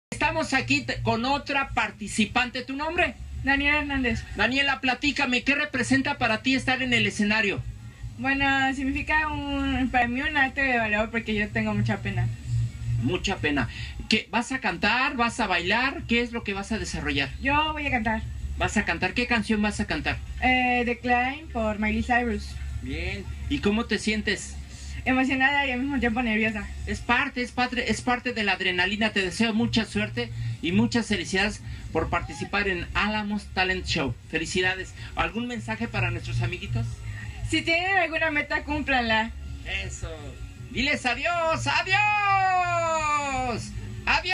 ALAMOS TALENT SHOW
ORI ENTREVISTA A DANIELA HERNANDEZ PARTICIPANTE DE ALAMOS TALENT SHOW